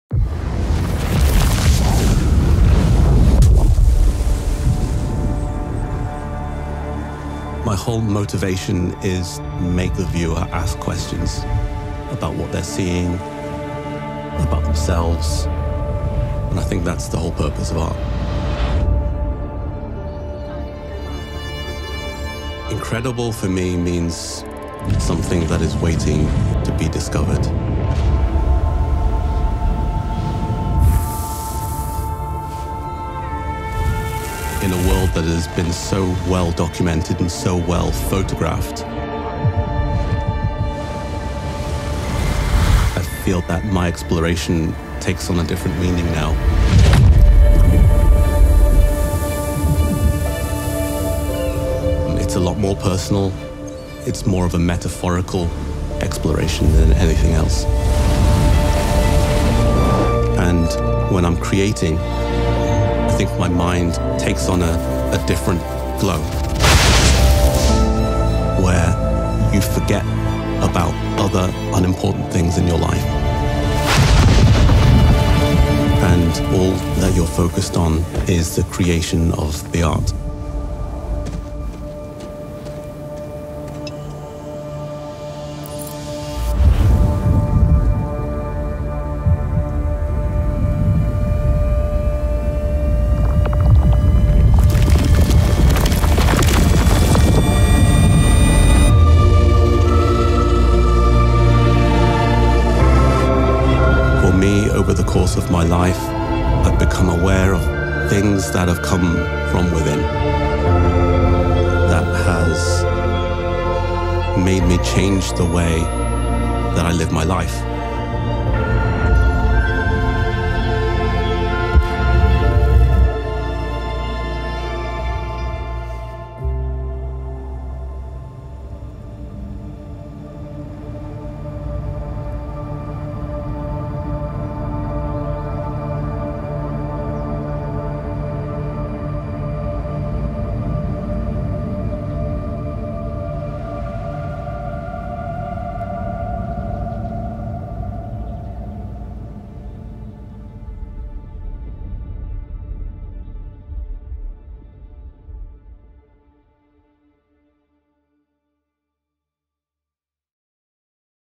Incredible Comes From Within - ASUS Zenbook Series | 2022
Renowned visual artist Reuben Wu features in 'Incredible Comes From Within' campaign that celebrates the brand sprit and design genesis for the 2022 ASUS Zenbook laptop lineup.

Incredible not only comes from within artists like Reuben, but it also comes from within each and every one of us. With drive and determination and an ASUS Zenbook, we can all find inspiration to unlock our creative powers within and create incredible moments of our own.